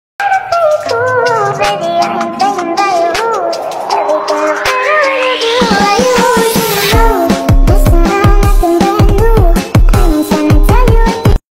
The face of this Alpaca looking at food is amazing
You can't help laughing when you see the face of this Alpaca looking at food.